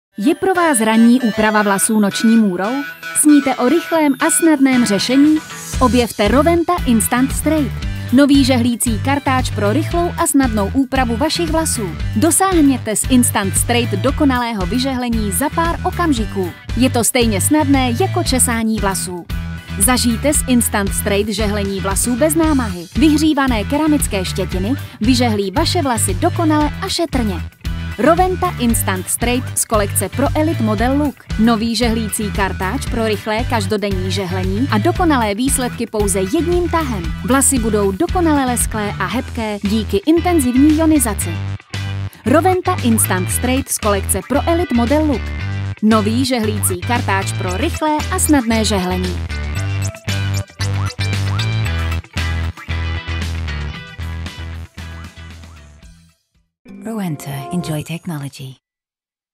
Rowenta CF5712 Instant Straight | Dokonalé narovnání a lesklé vlasy jedním tahem | Rowenta Česko
Ionizační žehlicí kartáč Rowenta Instant Straight vlasy rozčeše v jednom tahu bez toho, že byste museli oddělovat jednotlivé prameny (tak jako když si žehlíte vlasy žehličkou), a vyžehlí nahřátými keramickými štětinami – je ideální pro každodenní používání, díky generátoru iontů dojde k eliminaci statické elektřiny a krepatění.

Navíc keramický povrch vlasy nepoškozuje, ale dopřává jim péči – váš účes dokonale rozzáří.

Optimální teplotu 210° C pro rychlé a dokonalé narovnání vlasů si zkontrolujete na digitálním displeji.

Elektrický kartáč Rowenta CF5712 Instant Straight